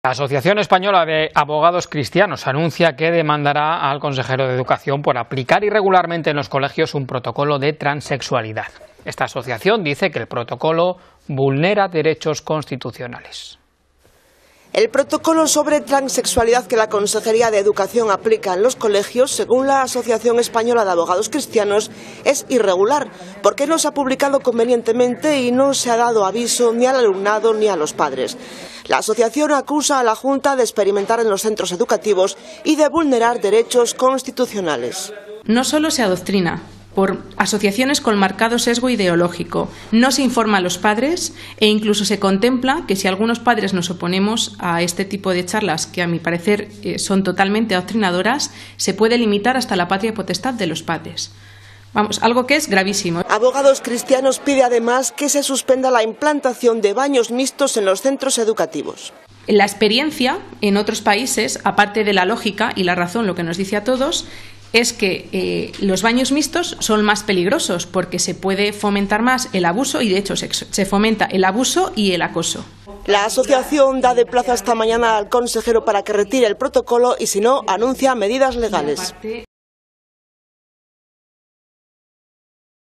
Polonia Castellanos anuncia medidas legales contra el Consejero de Educación de Castilla y León
La presidenta de Abogados Cristianos, Polonia Castellanos, ha anunciado en rueda de prensa una demanda contra el Consejero de Educación de Castilla y León por la aplicación de un protocolo de transexualidad que reúne numerosas irregularidades.